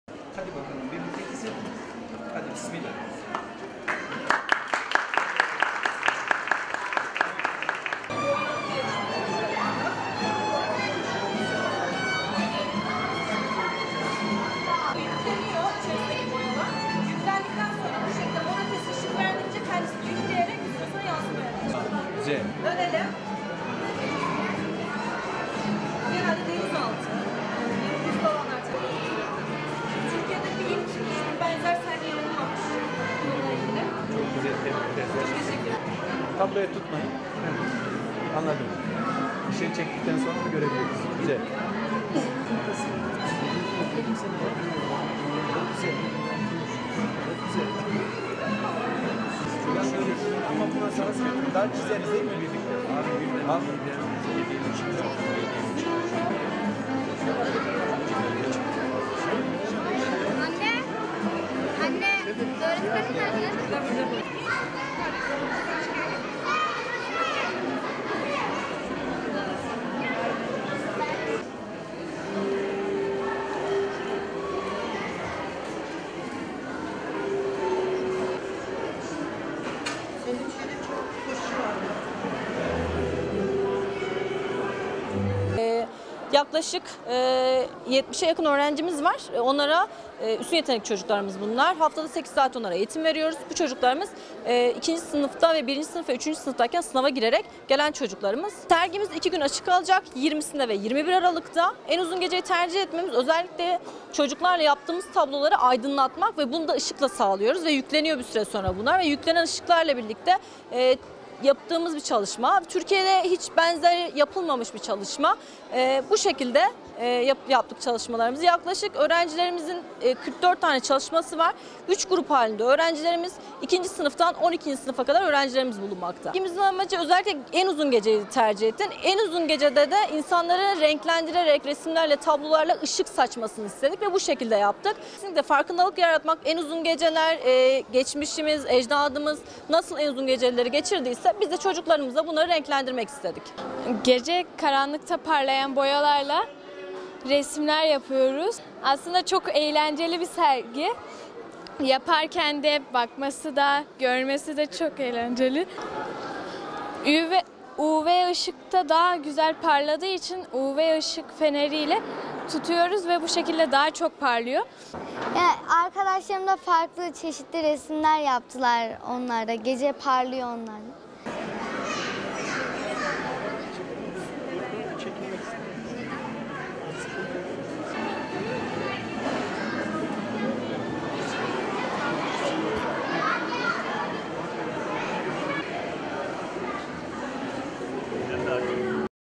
Karanlığa Renk Katan Muhteşem Fotoğraflar
Sivas Buruciye Bilim ve Sanat Merkezi’nde eğitim gören öğrenciler tarafından yapılan ve Ultraviyole ışık altında parlayan resimler ilgi gördü.